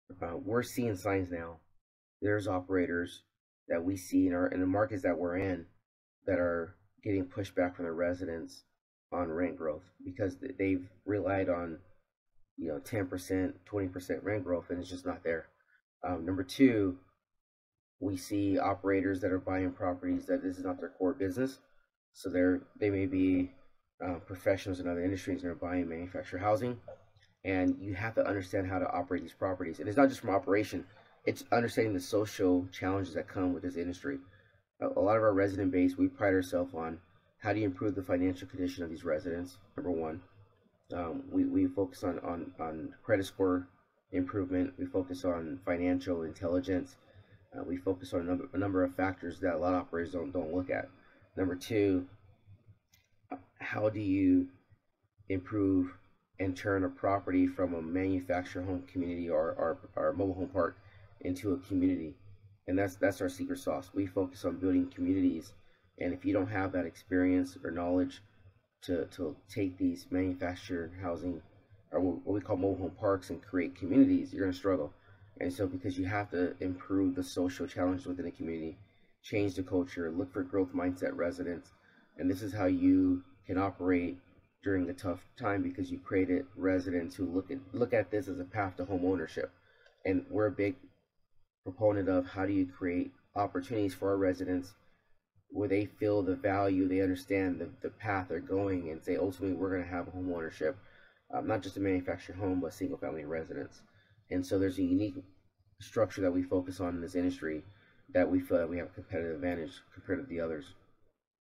Turning Mobile Home Parks into Thriving Communities
Rent growth should be coming from a place of trust and rapport. When a community is thriving, profits will just roll in.

Listen to our conversation with Demetre Booker on how they generate income by building thriving communities.